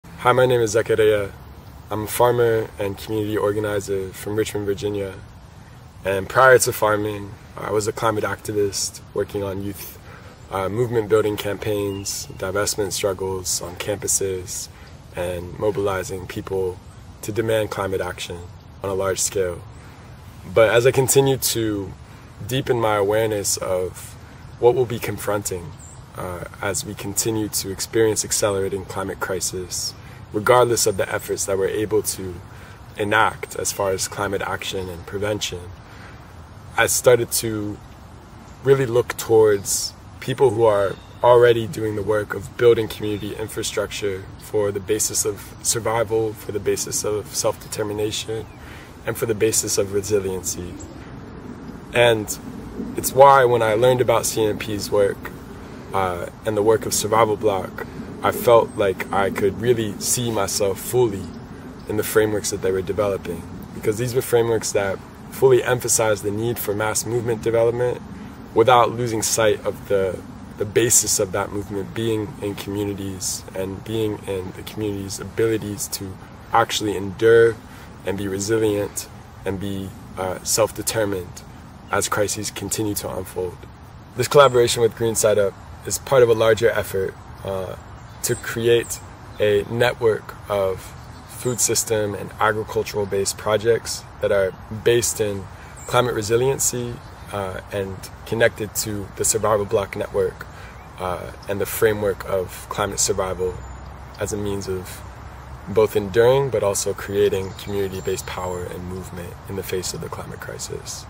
Zakaria reflects on farming & movement building
In 2024, Climate Mobilization Project will expand the role of farms in movement-building by turning farms in Richmond, VA and Las Vegas, NM into hubs for climate justice organizing, cooperative and solidarity economics, and skill-building around food sovereignty.